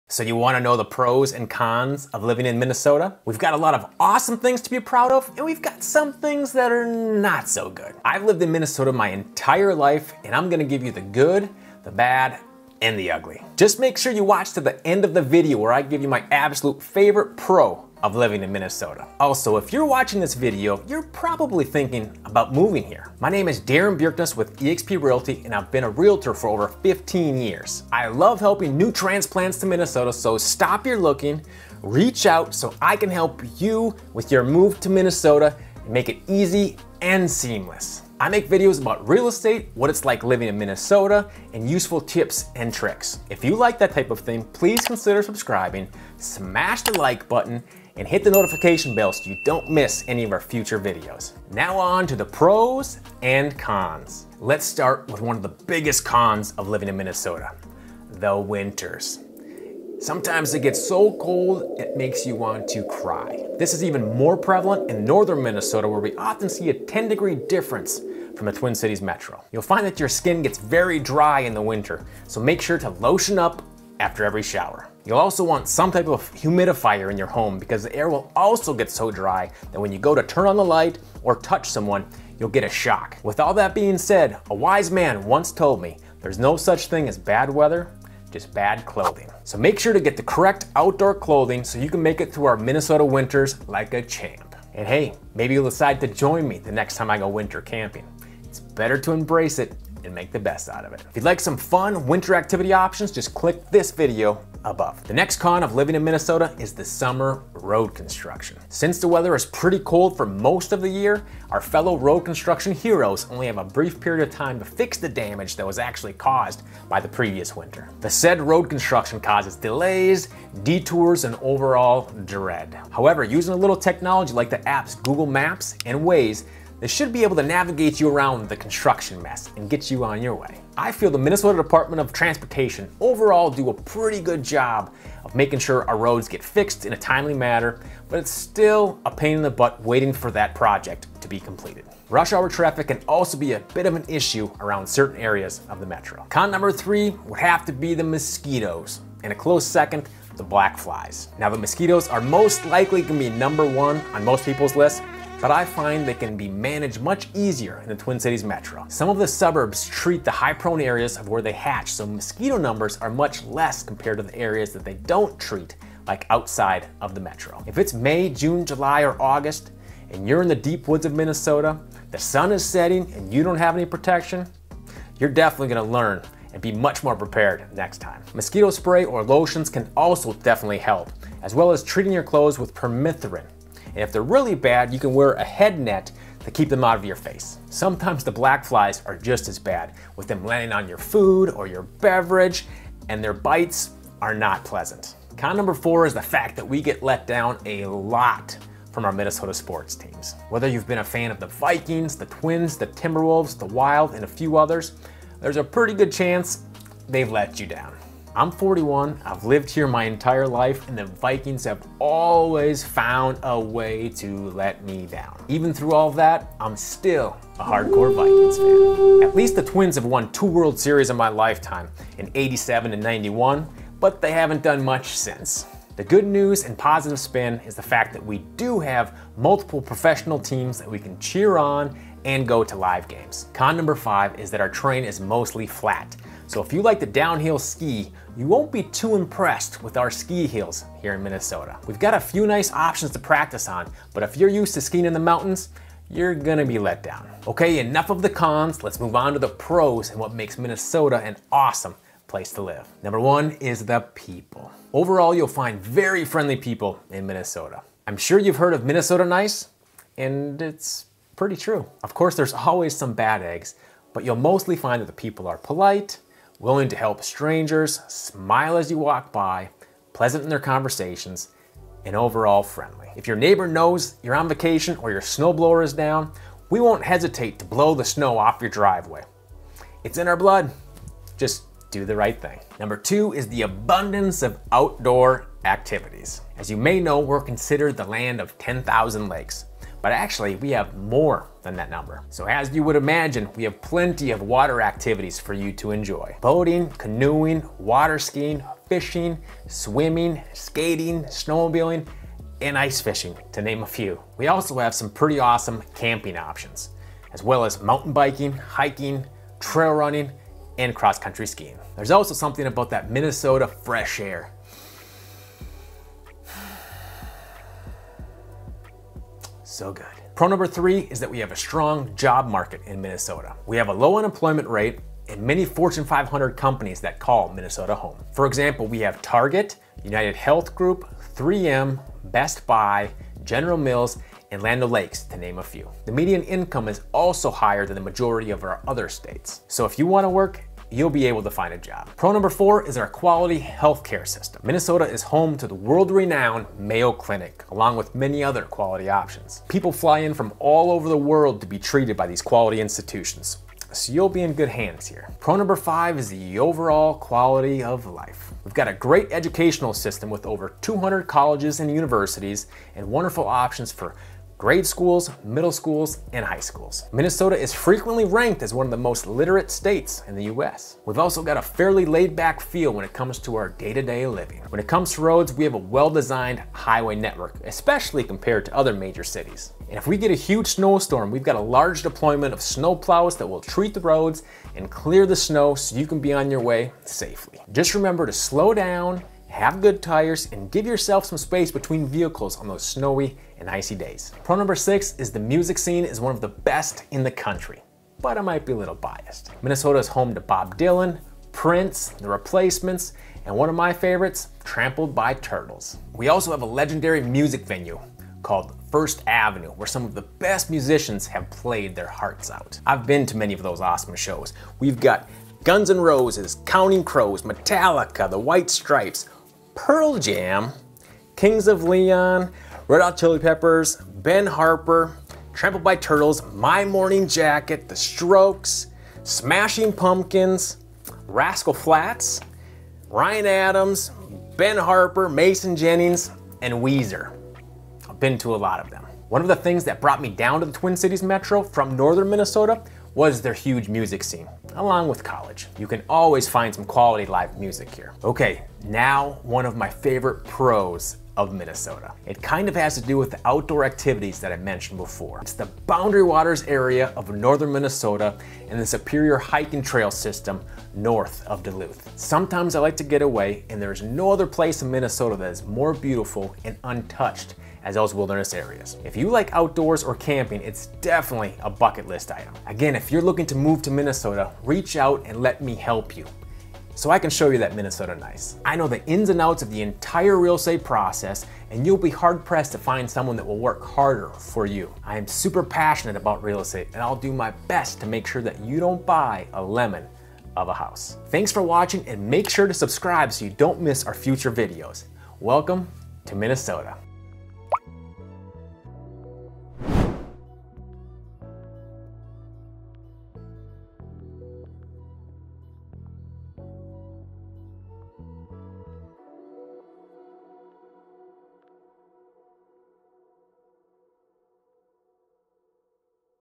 Living in Minnesota | Pros & Cons
So you want to know the Pros & Cons of Living in Minnesota?  We’ve got a lot of awesome things to be proud of and we also have some things that are not so good.
Let’s start with one of the biggest Cons of living in Minnesota…the Winters…the temperatures get so cold at times that it makes you want to cry.
The next Con of living in Minnesota is the Summer Road Construction…since the weather is pretty cold for a good portion of the year our fellow Road Construction heroes only have a brief period of time to fix the damage that was caused by the winter.
Con number three would have to be the Mosquitos and a close second, the black flies.  Now the mosquitos are most likely going to be number one on most people’s lists, but I find they can be managed much easier in the Twin Cities Metro.  Some of the suburbs treat the high-prone areas where they hatch so the mosquito numbers are much less compared to areas that they don’t treat like areas outside of the metro.
Con number 4 is the fact that we get let down a lot from our Minnesota Sports teams… whether you’ve been a fan of the Vikings, Twins, Timberwolves, Wild, and a few others…there’s a good chance that you’ve been let down.
Con number 5 is that our terrain is mostly flat and if you like to downhill ski you won’t be too impressed with our ski hills in Minnesota.  We’ve got a few nice options to practice on, but if you’re used to skiing in the mountains, you’re going to be let down.
Okay, enough of the Cons…let’s move on to the Pros and what makes Minnesota an awesome place to live!
Number 1 is the people…Overall, you find very friendly people in Minnesota. I’m sure you’ve heard of Minnesota nice…and it’s pretty true.  Of course, there are always some bad eggs, but you’ll mostly find that people are polite, willing to help strangers, smile as you walk by, pleasant in their conversations, and overall friendly. If your neighbor knows you’re on vacation or your snowblower is down they won’t hesitate to blow your driveway for you.
Pro number two is the abundance of outdoor activities.  As you may know, we are considered the land of 10,000 Lakes, but we actually have more than that number.  So as you would imagine we have plenty of water activities for you to enjoy.  Boating, canoeing, water skiing, fishing, swimming, skating, snowmobiling, and ice fishing to name a few. We also have some pretty awesome camping options as well as mountain biking, hiking, trail running, and cross-country skiing.  There is also something about that fresh Minnesota air.
Pro number three is that we have a strong job market in Minnesota.  We have a low unemployment rate and many Fortune 500 Companies that call Minnesota home.  For example, we have, Target, United Health Group, 3M, Best Buy, General Mills, and Land O’ Lakes to name a few.  The median income is also higher than the majority of our other states.  So if you want to work you’ll be able to find a job!
Pro number four is our Quality Health Care System.  Minnesota is home to the world-renown Mayo Clinic along with many other quality options.  People fly in from all over the world to be treated by these quality institutions, so you’re in good hands here.
Pro number 5 is the overall Quality of Life here.  We’ve got a great educational system with over 200 colleges and universities and wonderful options for grade school, middle schools, and high schools.  Minnesota is frequently ranked as one of the most literate states in the US. We’ve also got a fairly laid-back feel when it comes to our day-to-day living.  When it comes to roads we have a well-designed highway network especially compared to other major cities…and if we get a huge snowstorm, we’ve got a large deployment of snowplows that will treat the roads and clear snow so you can be on your way safely.  Just remember to slow down, have good tires, and give some space between vehicles on those snowy or icy days.
Pro number 6 is the music scene is one of the best in the country.  Minnesota is home to Bob Dylan, Prince, The Replacements, and one of my favorites Trampled By Turtles.  We also have a legendary music venue First Avenue where some of the best musicians have played their hearts out.
Okay, now for one of my favorite pros of Minnesota.  It kind of has to do with the outdoor activities that I mentioned before…It’s the Boundary Waters Area of northern Minnesota and the Superior Hiking trail system north of Duluth.

Darin Bjerknes
Minnesōtan
eXp Realty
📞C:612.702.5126

If you'd like help with buying or selling, I would be honored to take great care of you and your real estate needs.